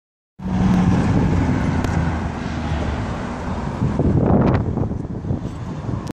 White Chevrolet C6 Corvette
Just missed the Alfa Romero 4C and BMW F80 M3 combo. Therefore the next best thing is a Chevrolet Corvette driving by. Immediately afterwards a BMW F80 M3 Competition Pack arrived. That's my next video.